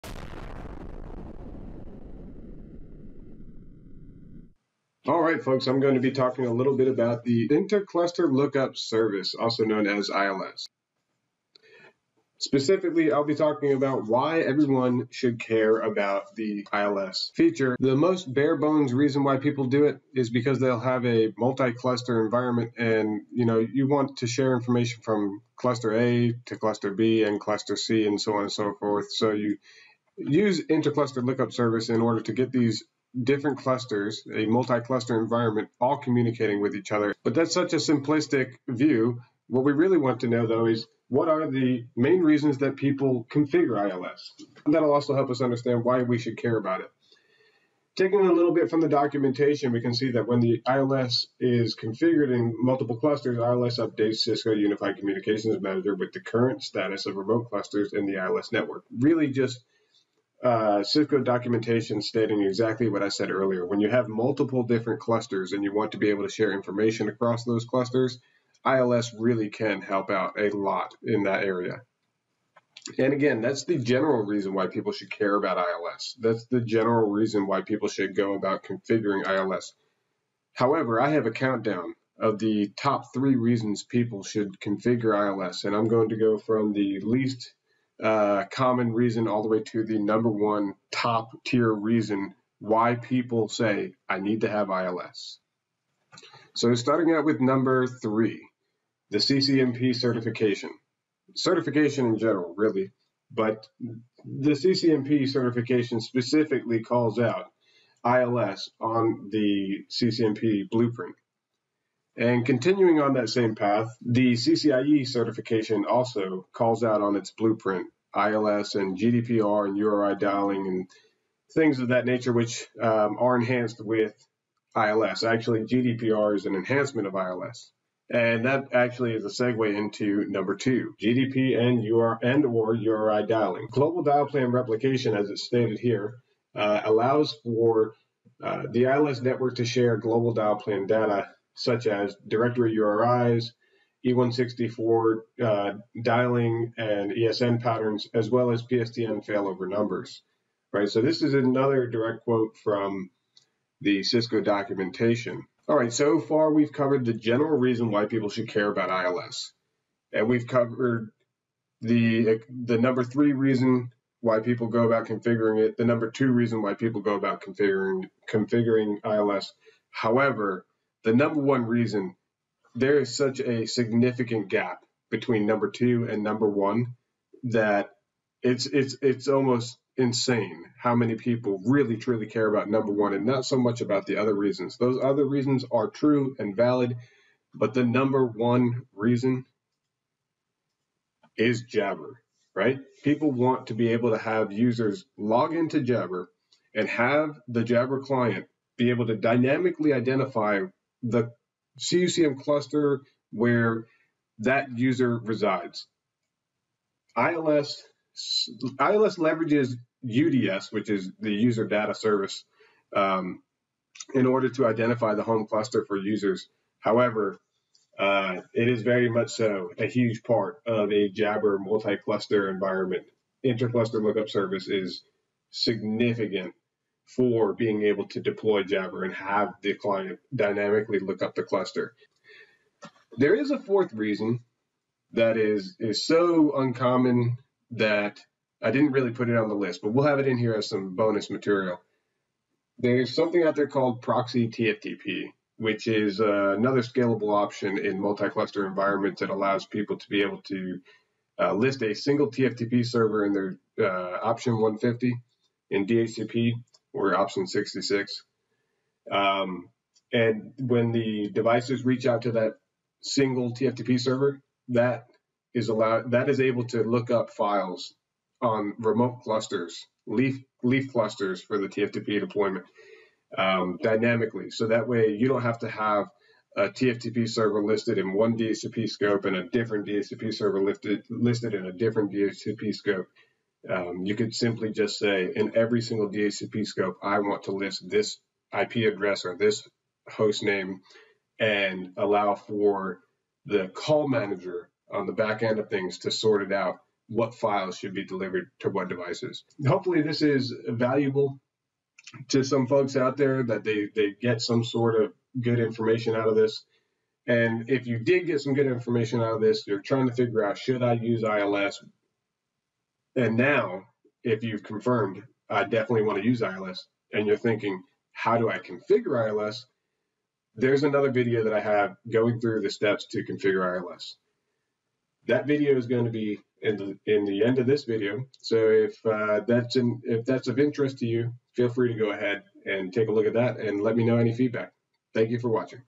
Why do people use the Intercluster Lookup Service (ILS)
Here are 3 reasons people care about ILS. The reasons that drive them to configure the feature.